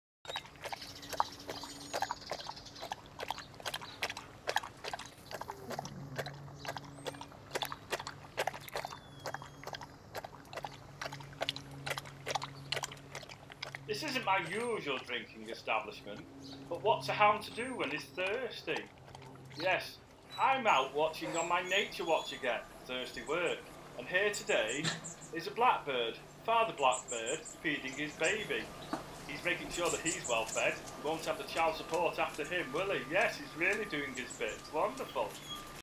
Nature Watch with Boris Basset Part 4
Boris Basset wets his whistle while watching a male Blackbird feed his baby.